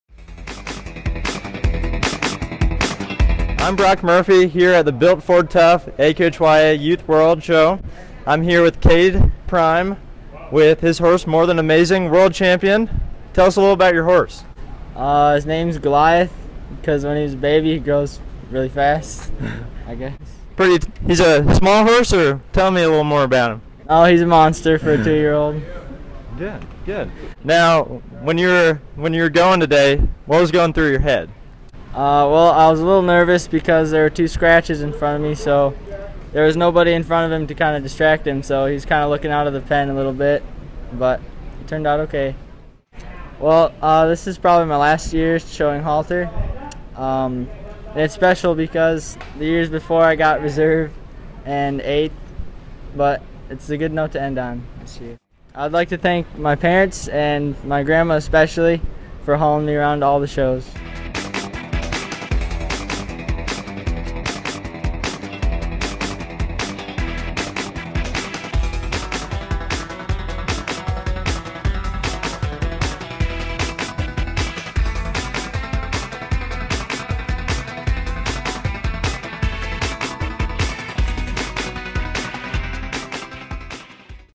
2017 AQHYA 2-Year-Old Geldings
Morthanamaizing and Cade Prime win 2-year-old geldings.